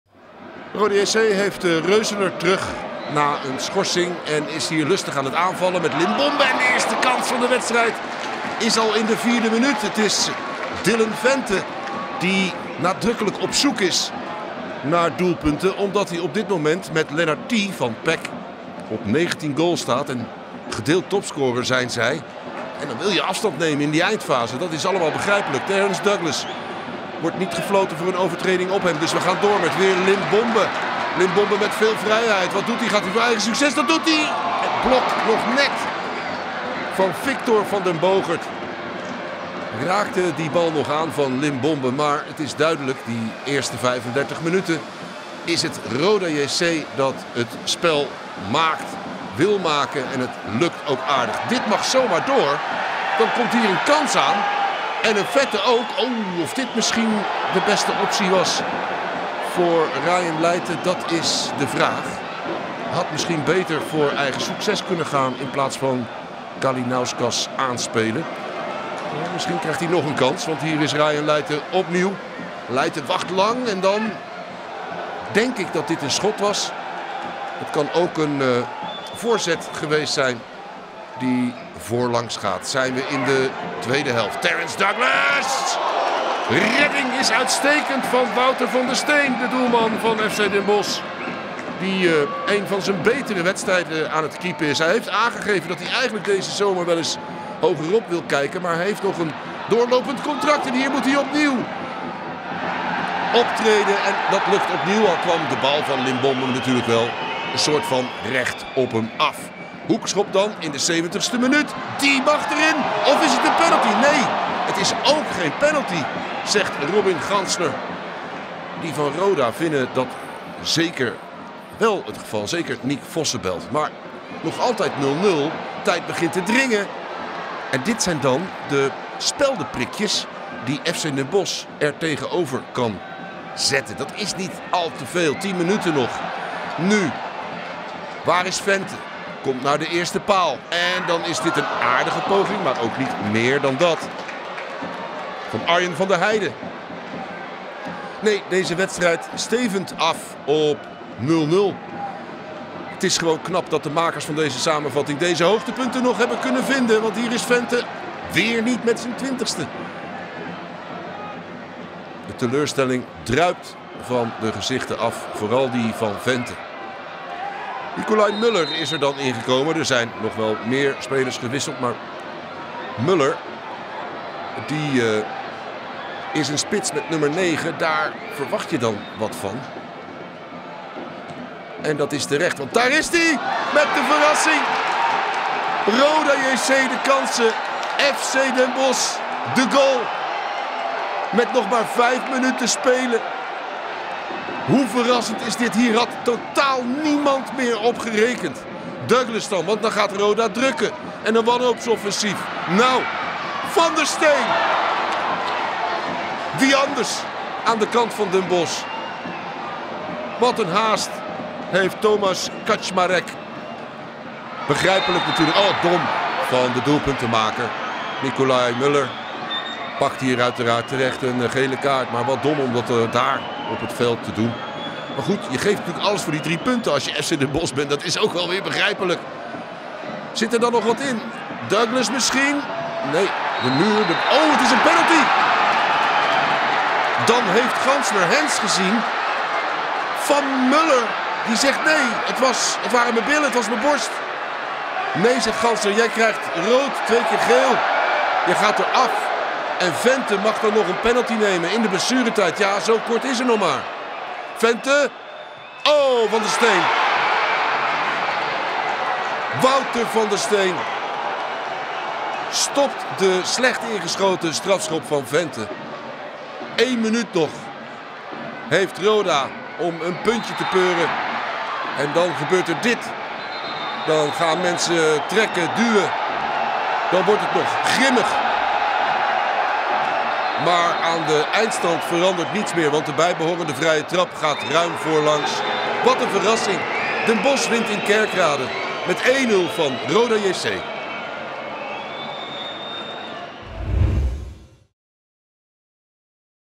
HIGHLIGHTS | Roda JC Kerkrade - FC Den Bosch | 21 april 2023
Bekijk hier de door ESPN samengestelde hoogtepunten uit de Keuken Kampioen Divisie-wedstrijd tussen Roda JC Kerkrade en FC Den Bosch. Het duel in Kerkrade eindigde vrijdag in een 1-0 verliespartij.